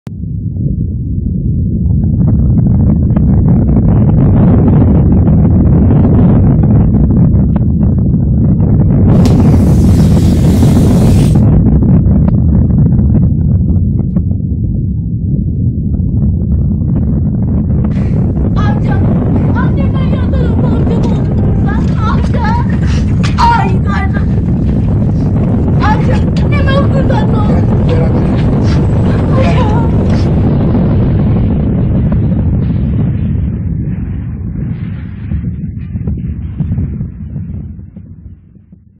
17 AĞUSTOS DEPREMİ
16 yıl önce bugün parçalandık ama unutmadık. Siz de unutmayın. Olası bir depremde yaralarımızı sarmak bizim elimizde.